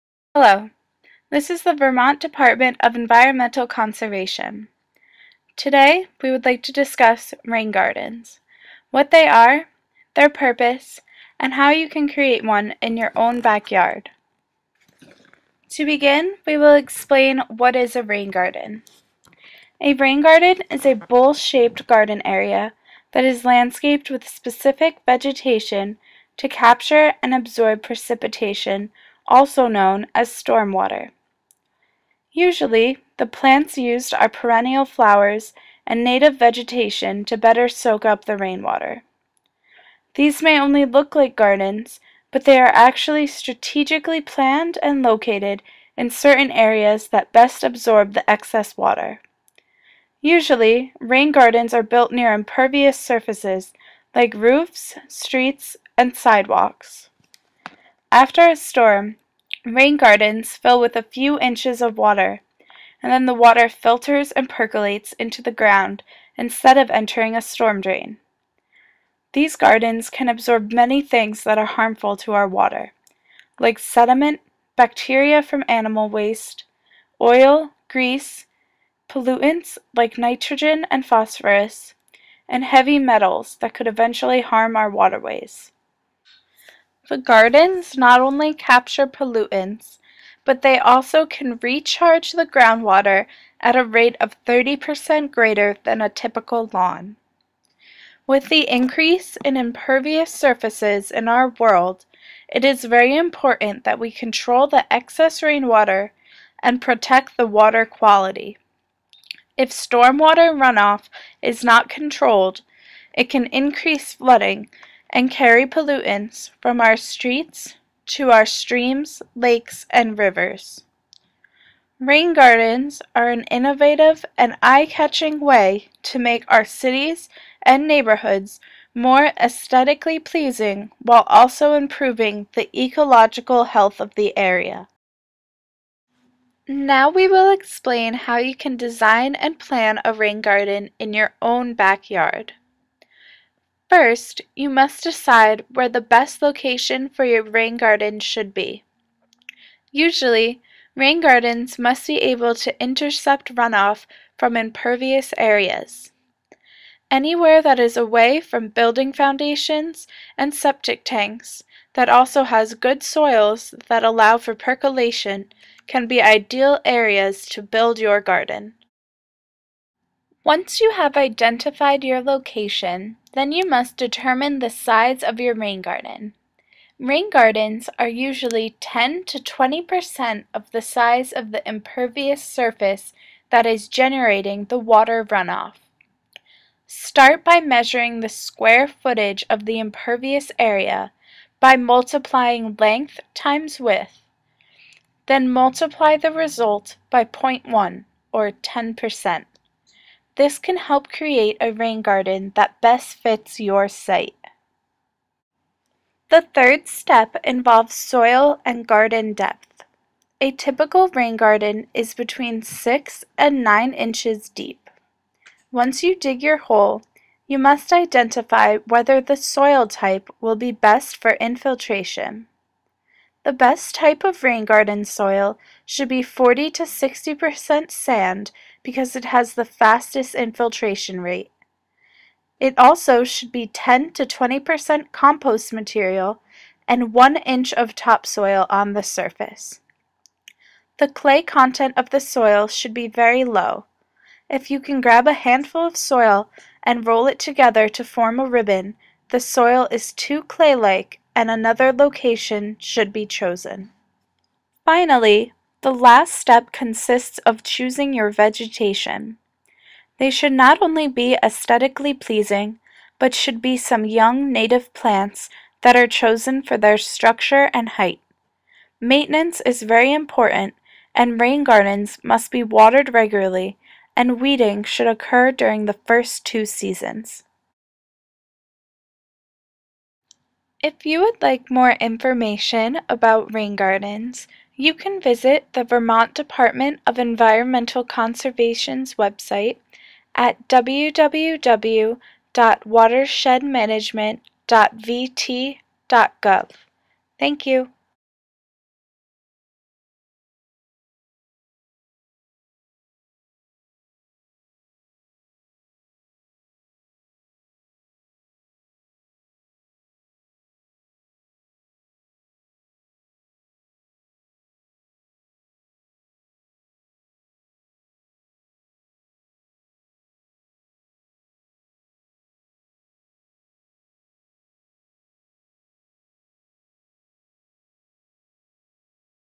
What is a Rain Garden?
Produced for the Vermont Green Infrastructure Initiative by Erin Schultz and Matthew Comrie as part of a project-based service-learning course at the University of Vermont.